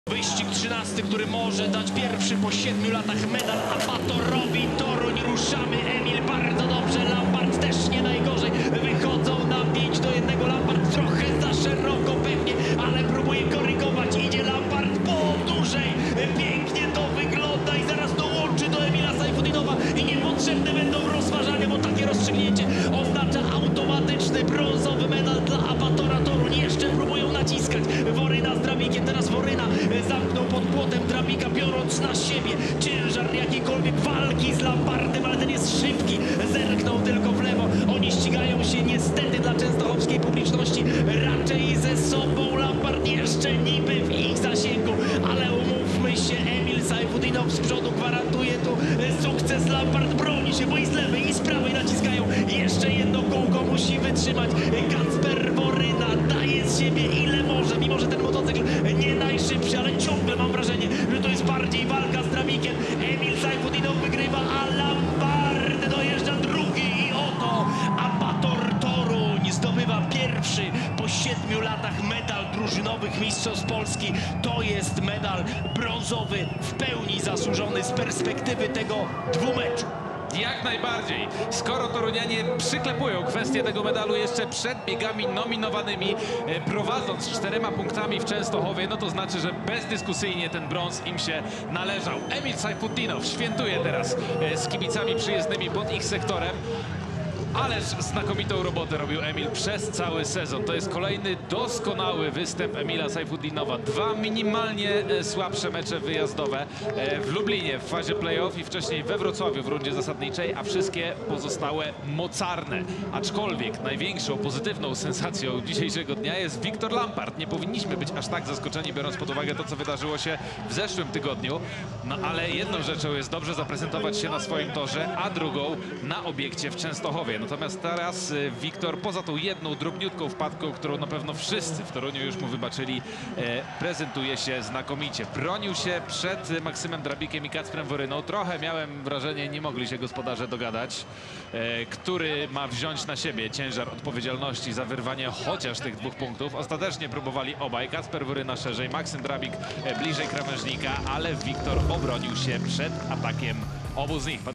BIEG O BRĄZ - TORUNIANIE ZDOBYWAJĄ MEDALE ZA SEZON 2023
W 13. biegu rewanżowego meczu pomiędzy Tauron Włókniarzem Częstochowa a For Nature Solutions KS Apator Toruń rozstrzygnęła się kwestia brązowych medali PGE Ekstraligi za sezon 2023.

Komentują Michał Korościel i Marcin Kuźbicki.